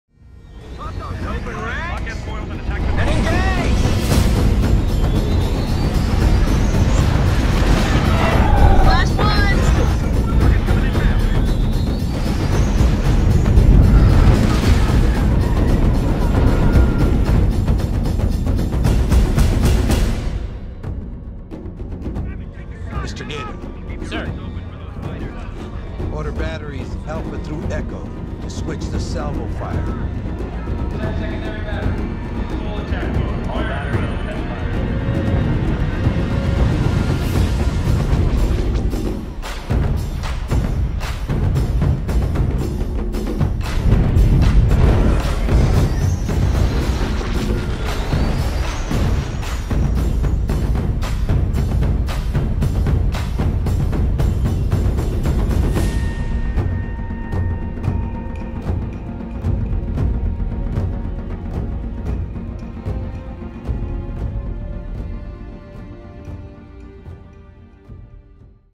[SFM] Rebellious [Star Wars Short Cinematic]
Originnaly a small idea for a sketch this unfortunnaly turned into this

The original Idea was to combine the Star Wars universe and the Battlestar Galactica Universe stylistically, in came movement, animations, sounds, ect. Its up to you to decide if I succeded
Assests used:

|MC-75/Venator Star Cruiser/Victory I Star Destroyer - belong to the Amazing Empire At War Remake mod Team |
|Human models from Battlefront 2 and Witcher - porter by ComradIvan|
|X-Wing and TIE Fitghter models by EA (Battlefront 2)|